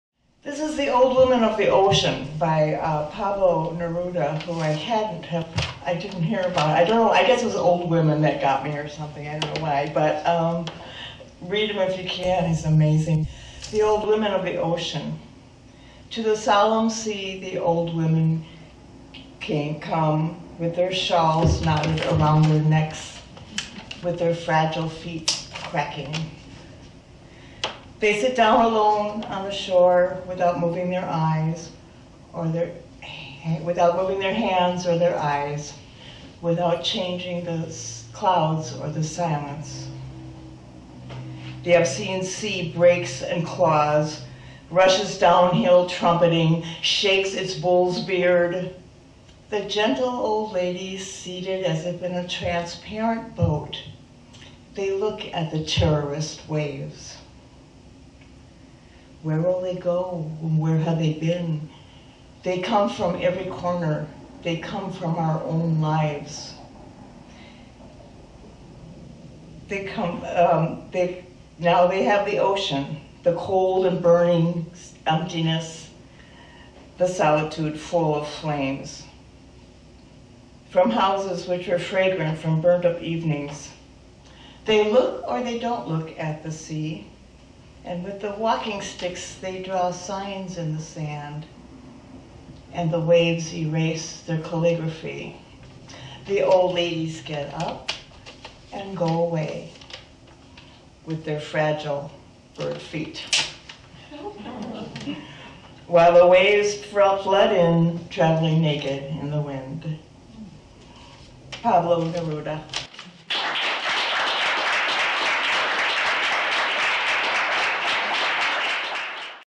Lynn Kuhns reciting "The Old Women of the Ocean" by Pablo Neruda
Feather & Bone Gallery inside The Draw
Appleton, Wisconsin